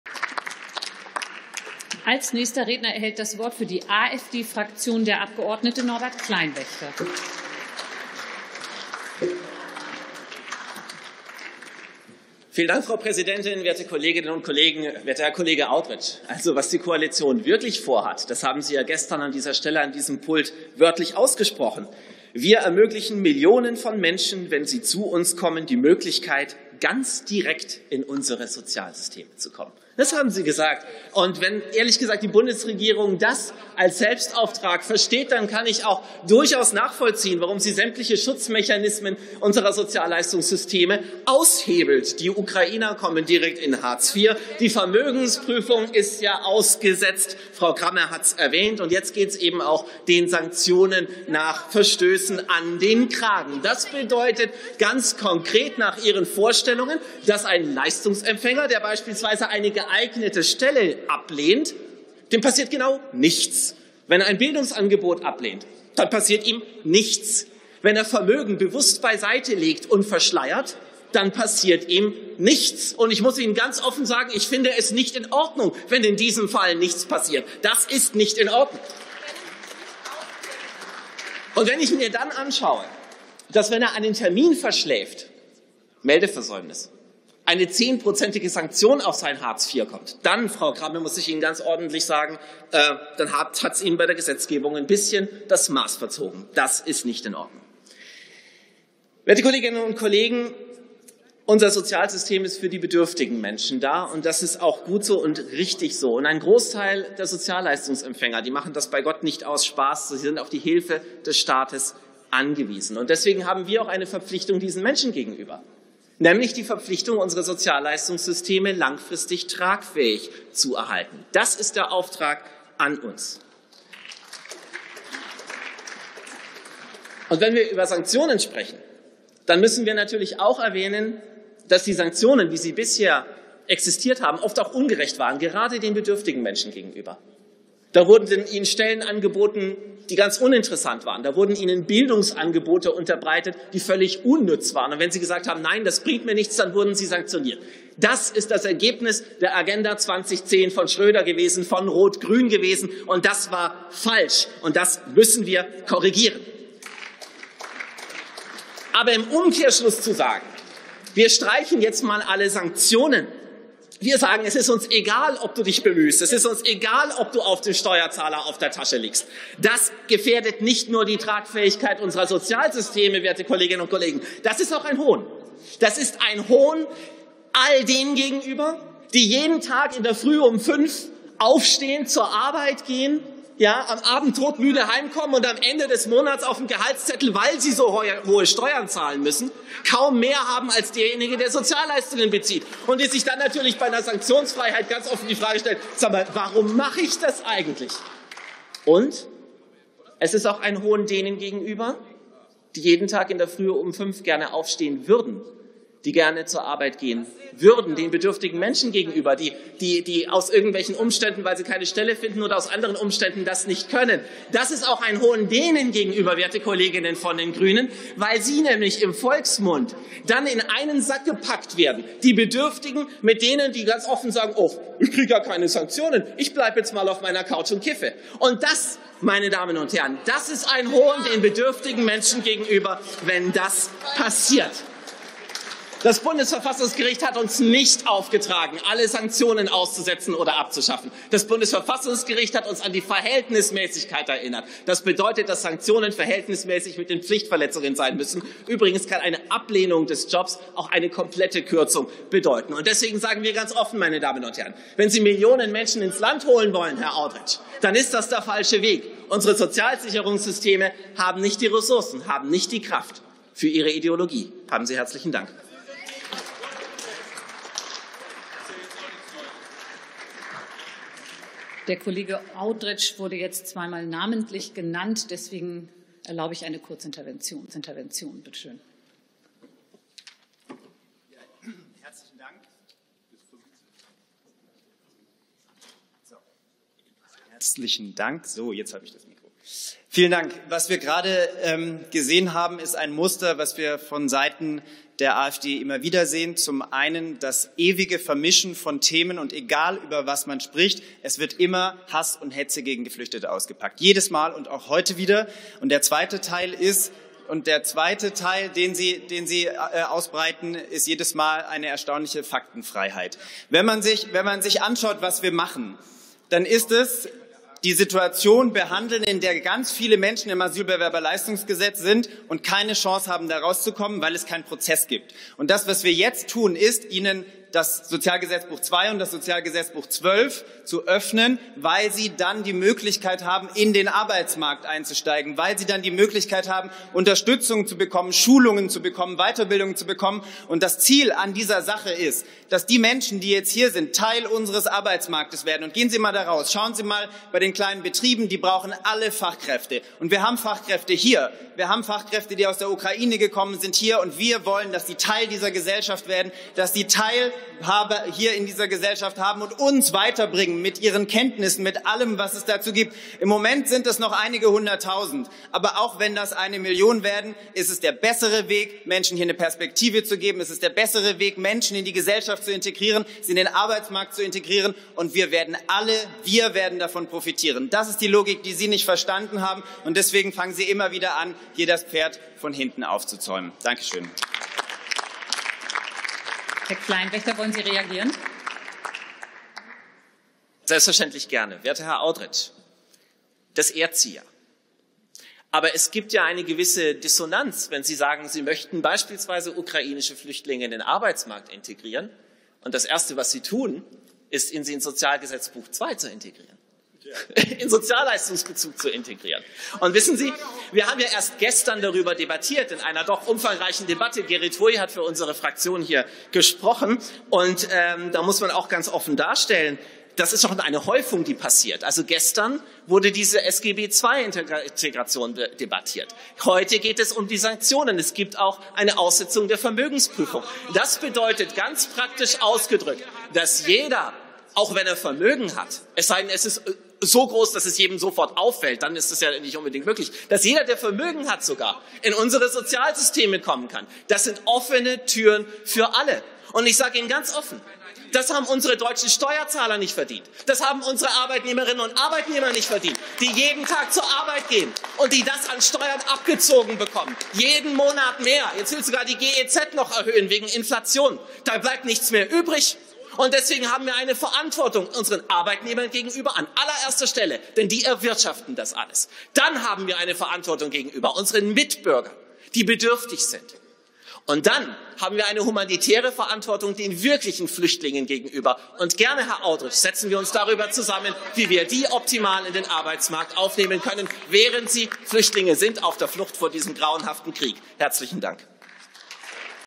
Millionen ins Sozialsystem - nicht mit uns!
Wahrhaft Bedürftigen muss geholfen werden, nicht Menschen die das System ausnutzen wollen!
Meine Rede zur Gesetzänderung im SGB II.